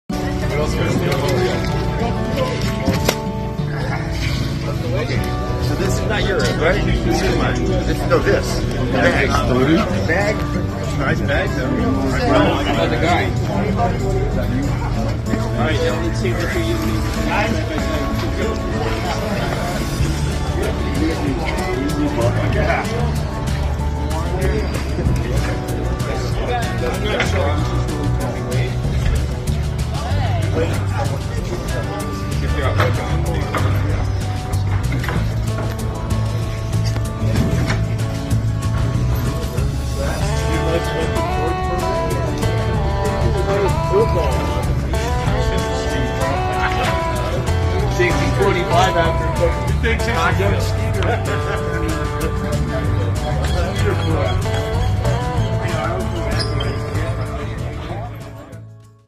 Huge Albacore and Blue Fin Tuna arrive early in the morning at Point Loma Harbor
I was at Point Loma Harbor in San Diego and an early fishing boat arrived with huge Albacore and Blue Fin Tuna